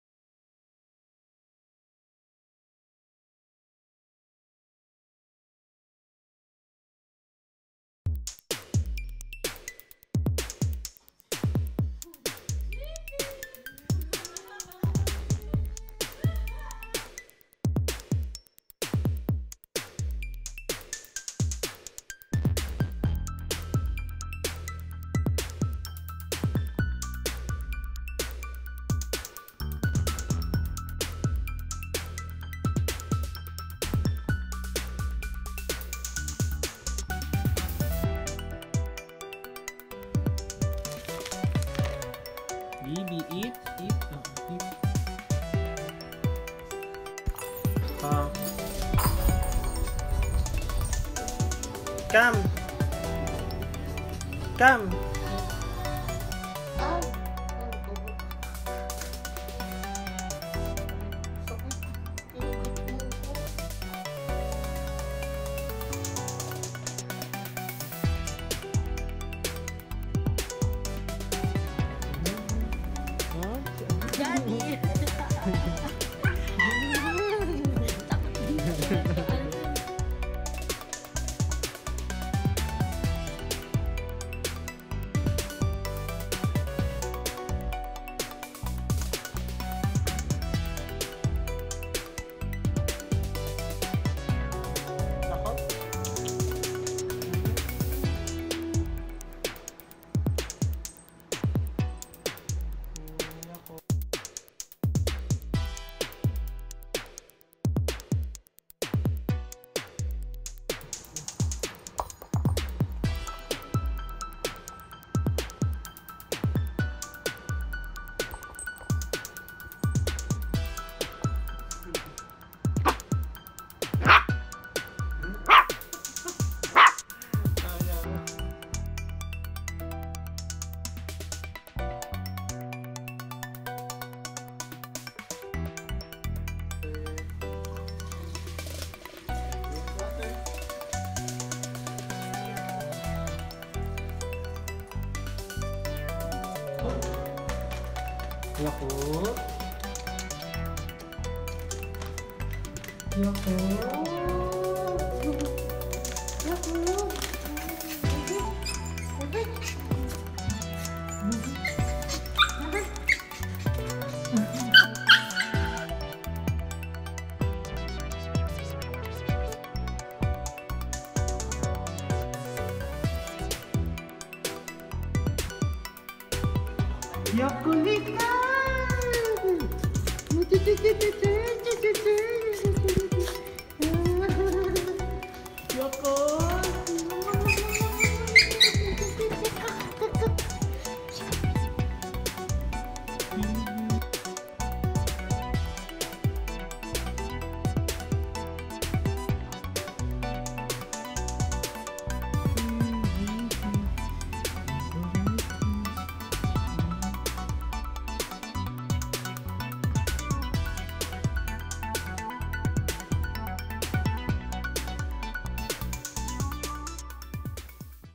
Cutest Hyperactive Maltese Ever! | Yakult The Maltese
Watch Yakult's compiled videos during her third month.